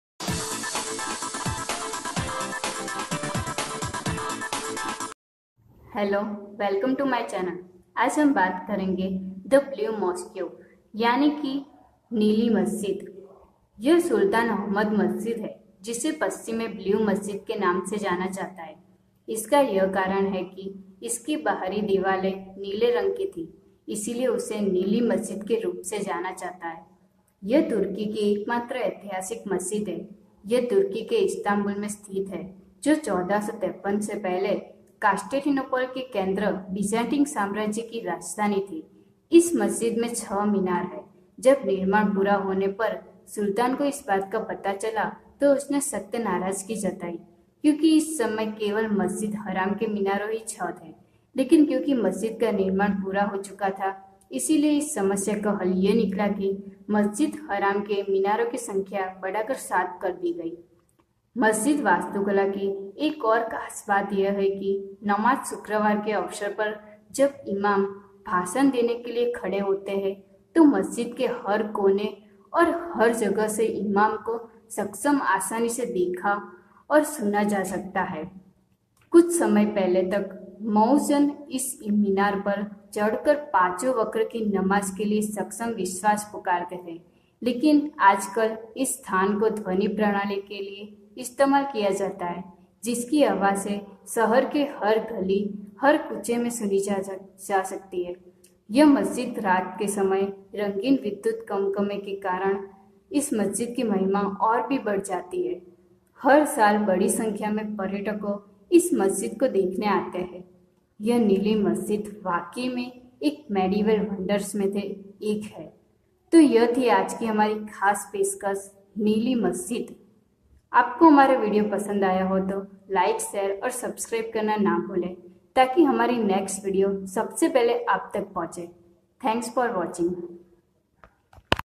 The blue mosque hindi || medieval wonder of world.
🎬My next video is the leaning tower of Pisa

               💯%important points 👇

👉 The Blue Mosque, one of the most important works of Ottoman history, was built by the architect, Mehmet Aga, at the request of Sultan Ahmet I. It is stated in many sources that Ahmed the First, who sat on the throne as the 14th Ottoman sultan at the age of 14 in 1603, was a highly religious person.

👉 Sultan Ahmed Mosque, known as theBlue Mosque by many tourists because of its bluish interior decoration, is the most important mosque of Istanbul standing next to the Byzantine Hippodrome in the old city center. ... When Ahmed I died in 1617, he was buried near the mosqueand a mausoleum was built over his tomb.

👉 It was constructed between 1609 and 1616 during the rule of Ahmed I. Its Külliye contains Ahmed's tomb, a madrasah and a hospice. Hand-painted blue tiles adorn the mosque'sinterior walls, and at night themosque is bathed in blue as lights frame the mosque's five main domes, six minarets and eight secondary domes.

👉 The Blue Mosque (or Sultanahmet mosque) is not one of the biggest but it's one of the most beautiful. It's called the Blue Mosque because of blue tiles on the walls. Hagia Sophiais a treasure from the Byzantine and Ottoman Empires. It was once a church, later a mosque, and now amuseum.

👉the  blue mosqeu age is 411(2020).